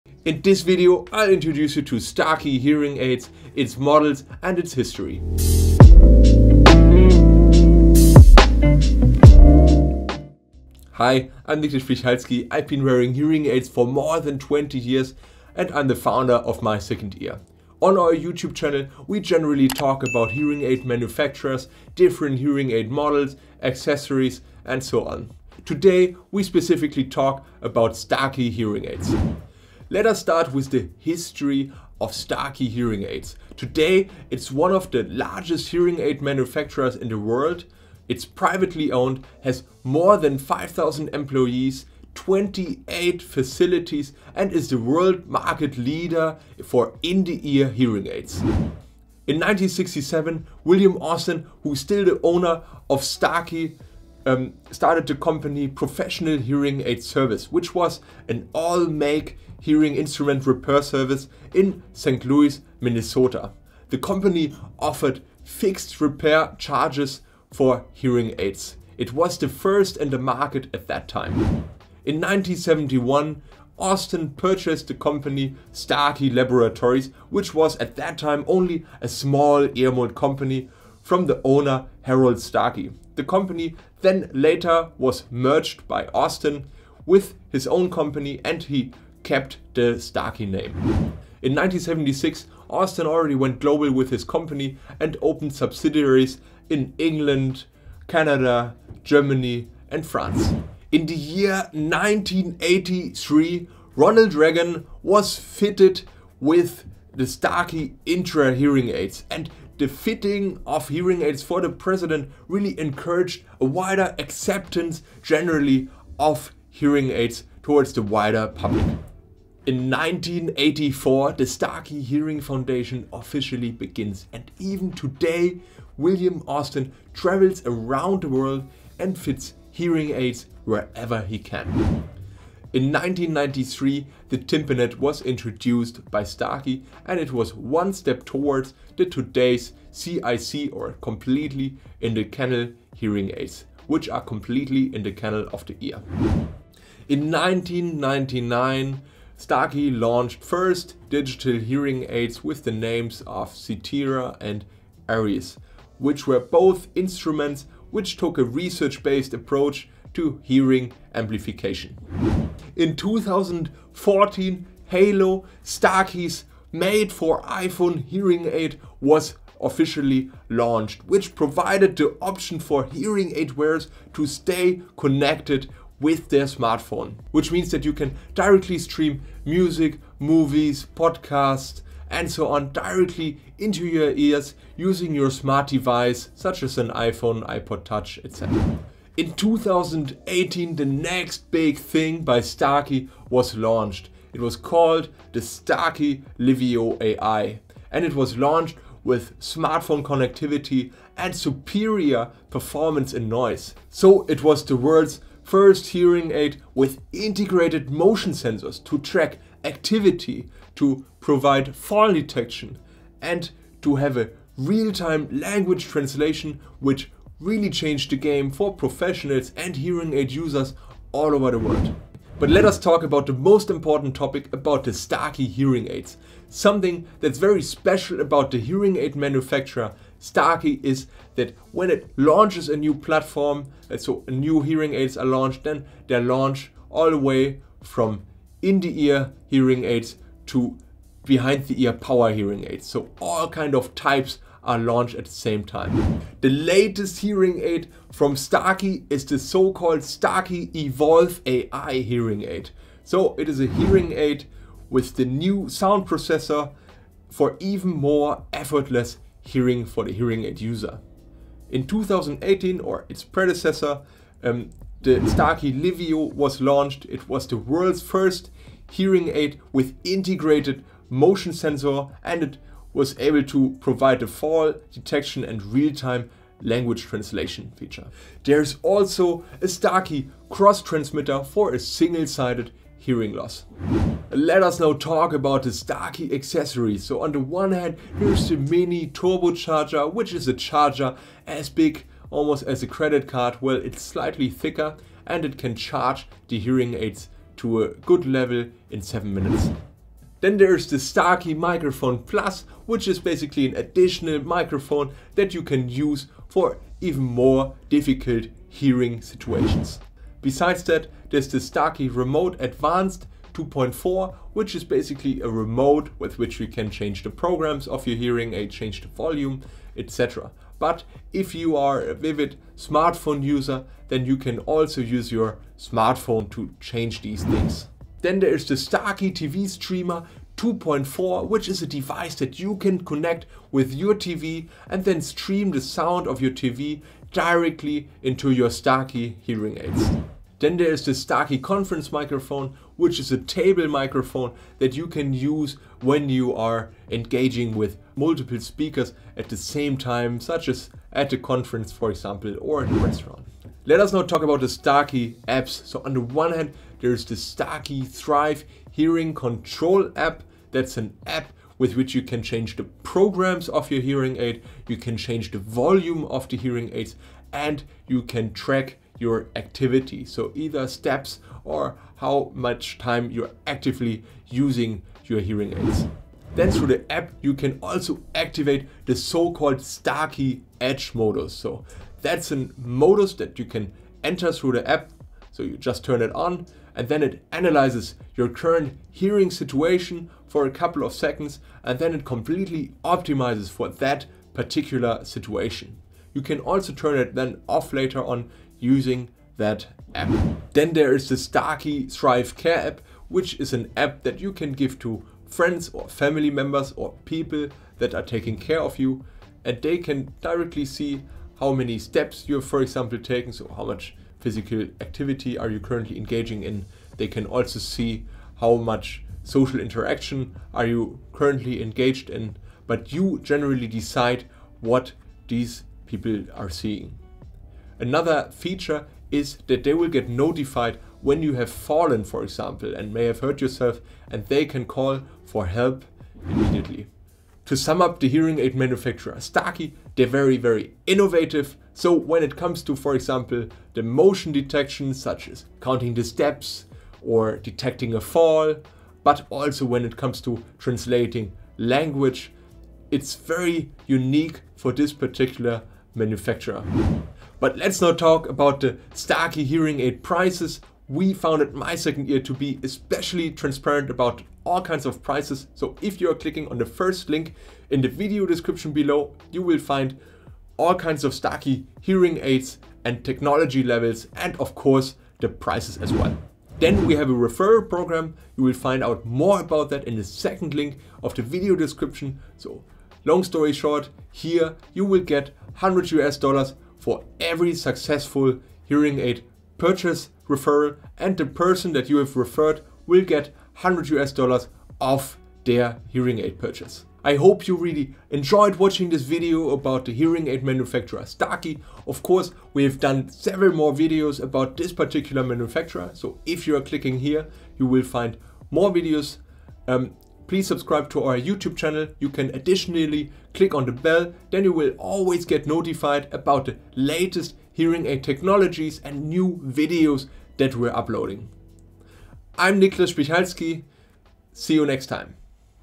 Starkey Hearing Aids: Models, Reviews, Prices 2024
Table of Contents:
00:00 Intro
00:48 Starkey History
03:44 Starkey Hearing Aid Models
04:42 Starkey Accessories
05:52 Starkey Apps
07:17 Summary
07:36 Starkey Hearing Aid Prices
08:17 Outro
Description:

The Company Starkey

Starkey is an American company based in Minnesota, which was founded in 1967 by William F. Austin. After this, Starkey originally focused on repairing hearing aids, however, soon they began the development of their own products. Starkey is now one of the world's largest manufacturers for hearing aids and has the uniqueness of being the only American hearing aid manufacturer with global reach. The company's base, as well as their research center, is still located in the USA. The company's international reach can be attributed to their innovative inventions. Starkey considers hearing our surroundings as essential for humans. Their focus is therefore always on creating access to optimal hearing for everyone via premium quality hearing aids.

Starkey Hearing Aids

Starkey hearing aids are available in a variety of styles and technology levels. You can opt between an ITE, as well as a BTE or RIC option. When choosing the technology level, the models vary between classes 1000 to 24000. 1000-models constitute the entry level with 10 adjustable channels and basic technical features. 1200- (basic) and 1600- (standard) models already offer 12 and 16 adjustable channels, and are convincing even in demanding listening situations. With the 2000- (mid-range) and 2400- (premium) versions, you will be able to skillfully master even very difficult listening environments.

Technology is Starkey's Strength

In recent years, Starkey has trumped with important technical developments. These include nanocoatings for protecting electronics and sensors, as well as the Pure Wave System and the Livio Al technology. These innovations have increasingly improved speech understanding. Starkey was also the first company to launch a special compacted version of ITE hearing aids in 1973. This type of hearing aid is placed directly in front of the eardrum, making them nearly invisible.

Thrive Hearing Control App for iPhone

The Thrive Hearing Control App allows you to control your hearing aids individually and adjust its settings manually. This refers to your music preferences, phone calls, navigation function, volume and much more. The app is even in possession of a location finder so you can never lose your hearing aids again. Additionally, if you use a Starkey Livio AI, the app can provide you with information about your fitness and health data. Likewise, within the app, you can set reminders and control the translation function. Finally, the app is also of importance for remote fittings, as these take place through the Hearing Control App.

Accessories from Starkey

TV Streamer - Stream audio from your TV directly through your hearing aids and never miss a movie night again!

Turbo Charger - Charge your hearing aid in 7 minutes with this portable charger and enjoy 3.5 hours of clear hearing!

Conference Microphone - Follow all conversations at the table or designate a speaker to focus the microphone at, if necessary.

Remote Microphone - Use the Remote Microphone for better speech understanding of the person you're talking to or connect it to your laptop for being able to follow video conferences better.

SurfLink Mini Mobile - Connect your hearing aids to your bluetooth devices via the SurfLink Mini.